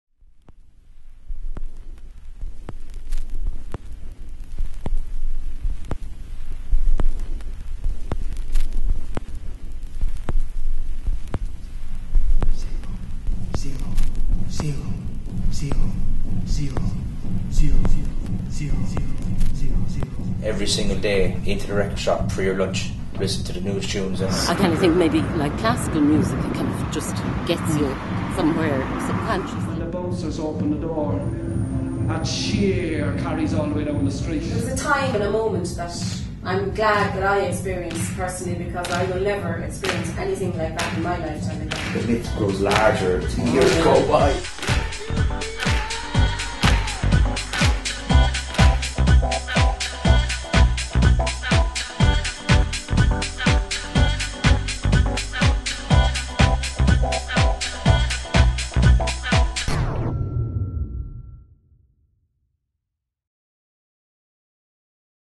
DEEP Tour 2014 Galway Promo
Deep by Raymond Scannell, Galway Arts Festival, July 2014, part of national tour 2014, funded by The Arts Council.
Deep - the evolution of the underground scene through the eyes of a vinyl junkie.